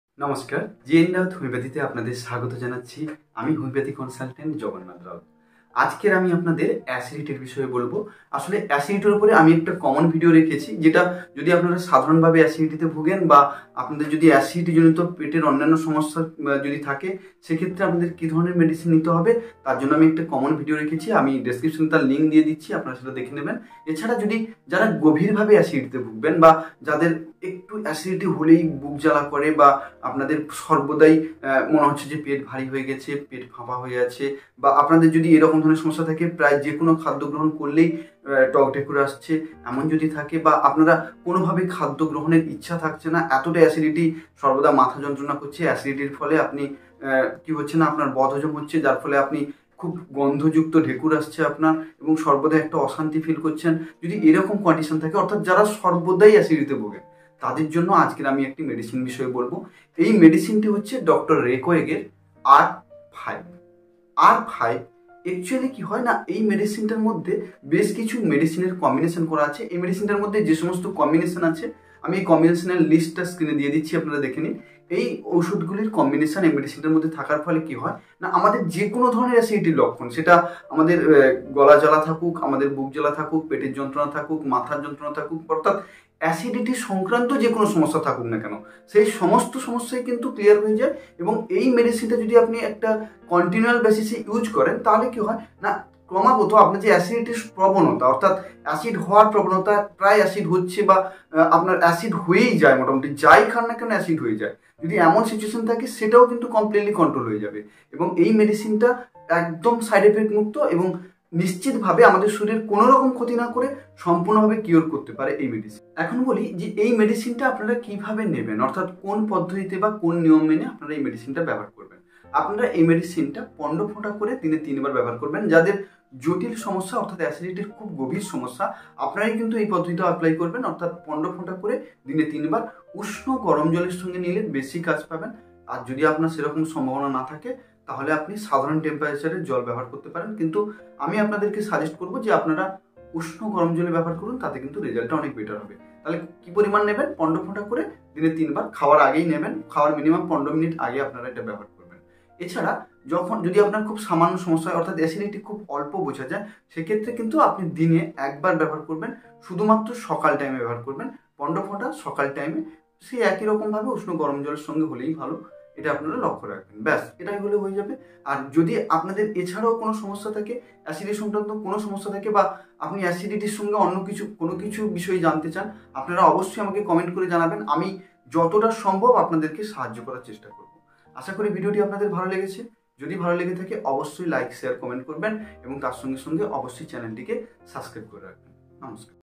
অ্যাসিডিটি চিকিৎসায় হোমিওপ্যাথি | Acidity
Hi friends, I am Jagannath. Watch this video to know about the treatment of acidity, acidity symptoms, acidity treatment, and R5.

Medicine :

R5 - 15 drops, 3 times a day with half cup of water.

                        ধন্যবাদ